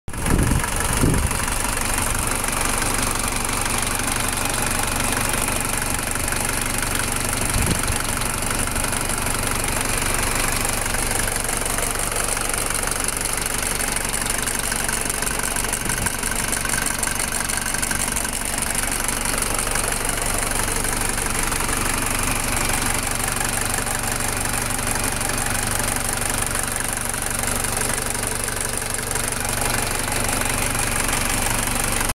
Engine Running in a 1985 Mercedes Benz 190D 2.2
Nice running engine in a 1985 Mercedes benz. It shows 305K miles on the clock!

190d, 2.2d, benz diesel